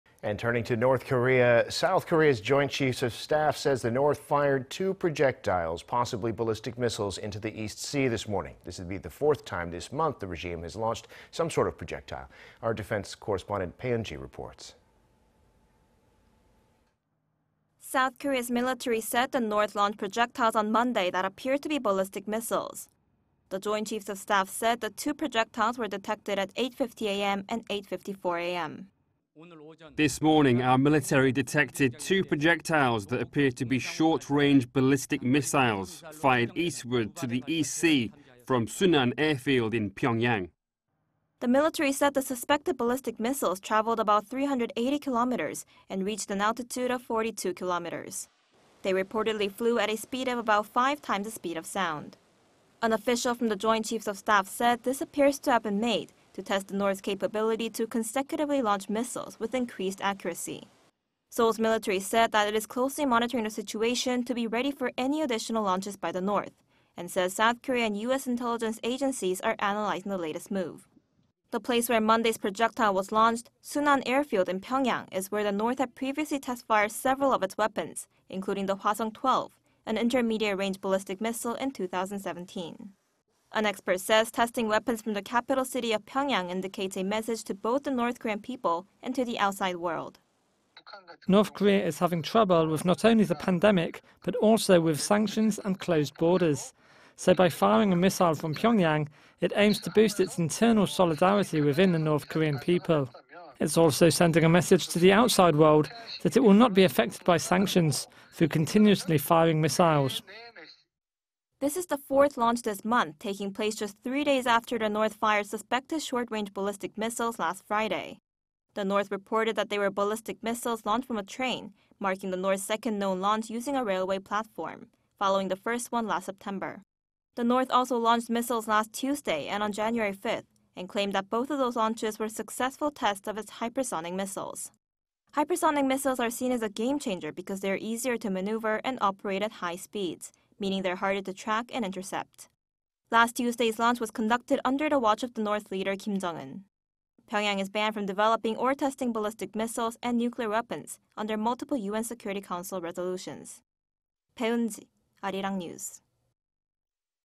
N. Korea launches suspected short-range ballistic missiles; 4th projectile launch this month
북, 평양 비행장→동해상으로 단거리 탄도탄 추정 2발 발사...새해 4번째 도발

Turning to North Korea, South Korea's Joint Chiefs of Staff said North Korea fired two projectiles, possibly ballistic missiles... into the East Sea this morning.
This is the fourth time this month that the regime has launched some sort of projectile.
Our defense correspondent Bae Eun-ji reports.
South Korea's military said the North launched projectiles on Monday that appear to be ballistic missiles.
The Joint Chiefs of Staff said the two projectiles were detected at 8:50 AM and 8:54 AM.

"This morning, our military detected two projectiles that appear to be short-range ballistic missiles,... fired eastward to the East Sea... from Sunan airfield in Pyeongyang."

The military said the suspected ballistic missiles traveled about 380 kilometers... and reached an altitude of 42 kilometers.
They reportedly flew at a speed of about five times the speed of sound.
An official from the Joint Chiefs of Staff said this appears to have been made... to test the North's capability to consecutively launch missiles, with increased accuracy.
Seoul's military said that it is closely monitoring the situation, to be ready for any additional launches by the North,... and said South Korea and U.S. intelligence agencies are analyzing the latest move.
The place where Monday's projectile was launched,... Sunan airfield in Pyeongyang... is where the North had previously test-fired several of its weapons, including the Hwasong-12,... an intermediate-range ballistic missile in 2017.
An expert says testing weapons from the capital city of Pyeongyang indicates a message to both the North Korean people and to the outside world.

"North Korea is having trouble with not only the pandemic,... but also with sanctions and closed borders. So by firing a missile from Pyeongyang, it aims to boost its internal solidarity within the North Korean people. It's also sending a message to the outside world that it will not be affected by sanctions,... through continuously firing missiles."

This is the fourth launch this month,... taking place just three days after the North fired suspected short-range ballistic missiles last Friday.
The North reported that they were ballistic missiles launched from a train,... marking the North's second known launch using a railway platform,... following the first one last September.
The North also launched missiles last Tuesday and on January 5th,... and claimed that both of those launches were successful tests of its "hypersonic" missiles.
Hypersonic missiles are seen as a game-changer because they are easier to maneuver and operate at high speeds... meaning they are harder to track and intercept.
Last Tuesday's launch was conducted under the watch of the North's leader Kim Jong-un.
Pyeongyang is banned from developing or testing ballistic missiles and nuclear weapons... under multiple UN Security Council resolutions.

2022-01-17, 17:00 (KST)